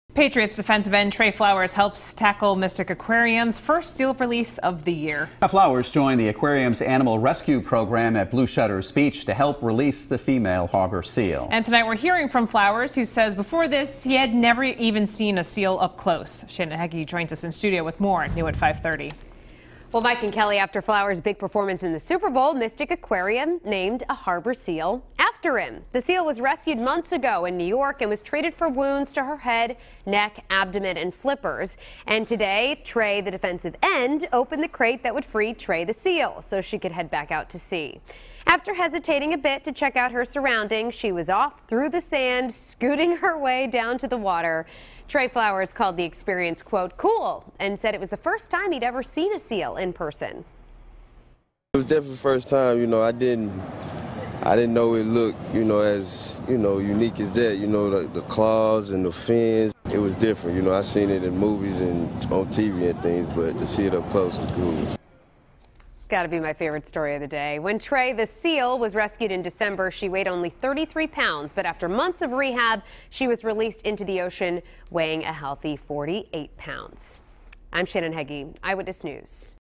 Patriot namesake harbor seal returns to sea after rehab stint
A seal named after Trey Flowers returned to the sea Tuesday with the help of the Patriots player and Mystic Aquarium.